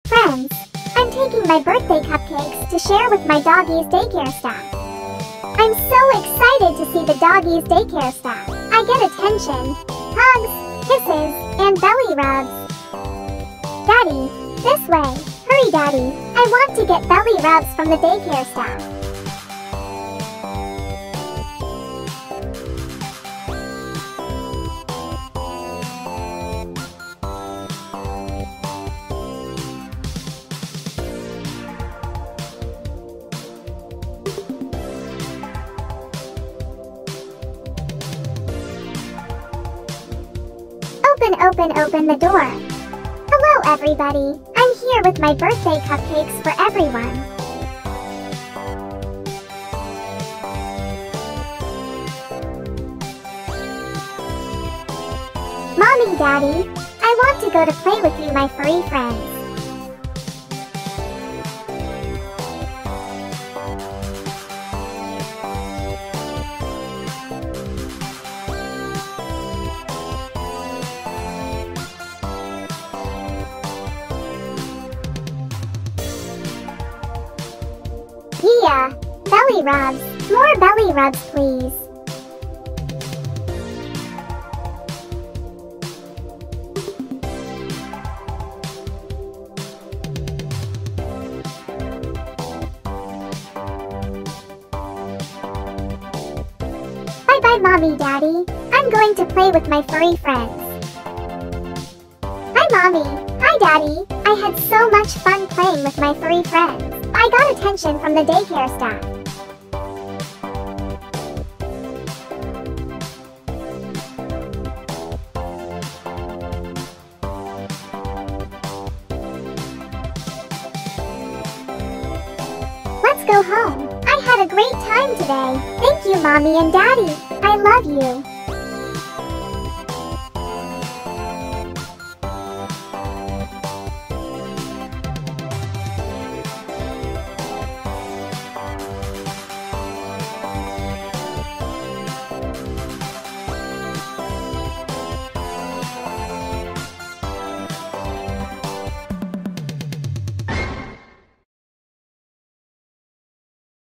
Bella’s Birthday Pawty - Part 4 | Bichon loves going to dog daycare
Bella brings the birthday cupcakes to the dog daycare to share with the daycare staff. Bella Bichon loves to get attention from the doggie daycare staff. Bella loves to get hugs, kisses, and belly rubs !

Bella Bichon is a sweet Bichon Frise dog who loves going to the doggies daycare to play with her furry friends :-)